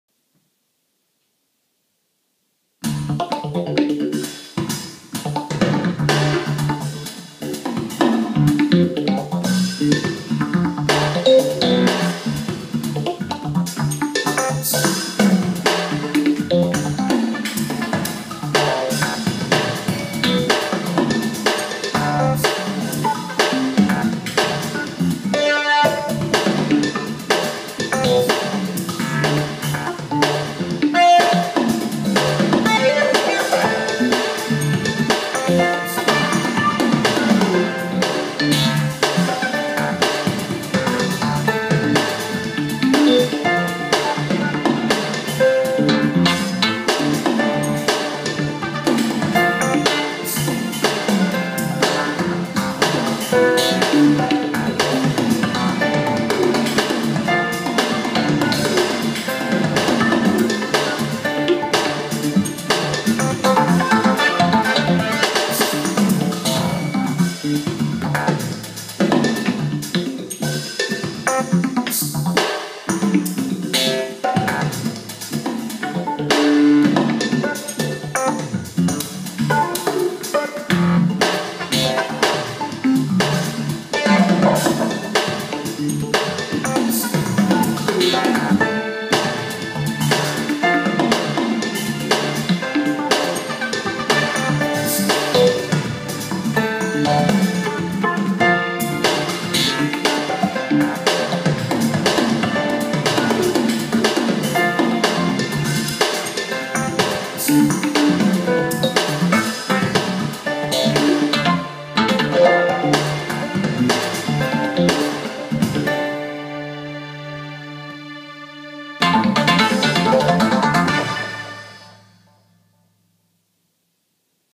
Old Dog New Trick
Performed live; no looping, no loops, nothing playing along, no auto accompaniment, no sequencing -- played live with just a pair of drumsticks.